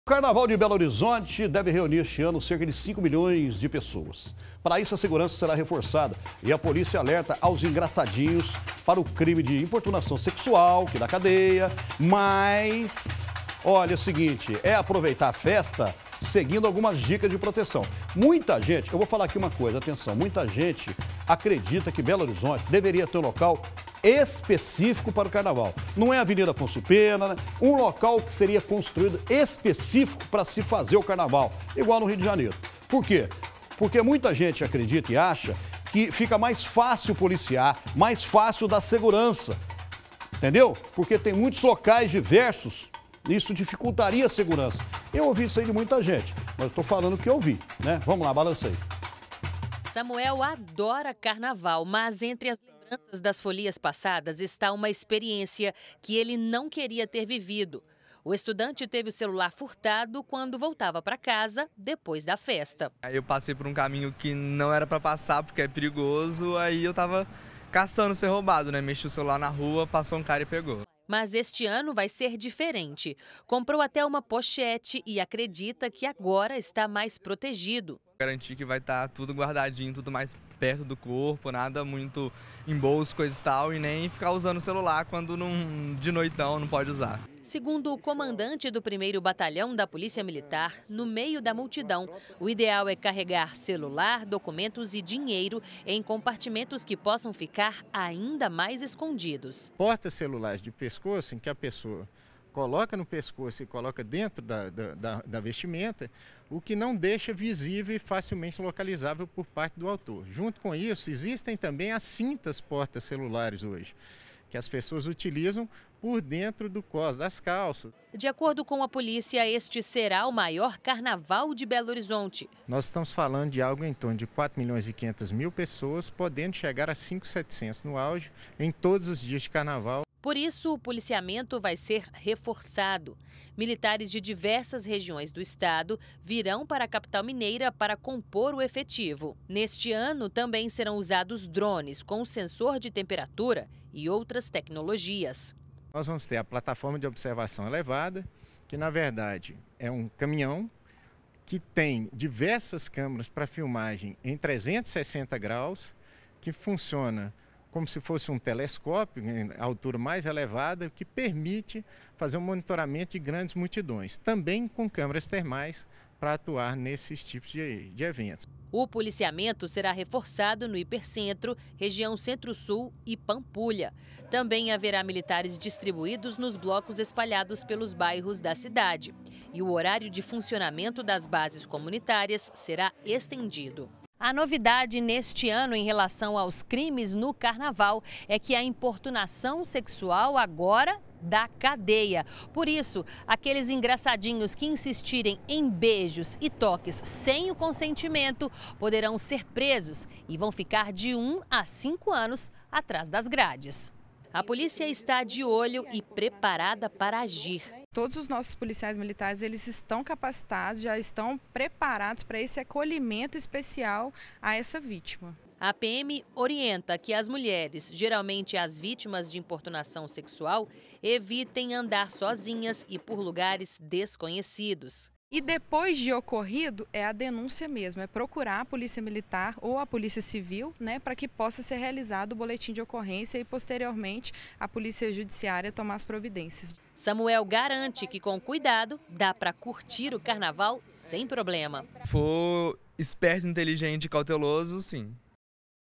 Polícia Militar reforça segurança no Carnaval de Belo Horizonte
O Carnaval de Belo Horizonte deve reunir, em 2019, cerca de 5 milhões de pessoas. Para isso a segurança será reforçada na capital mineira.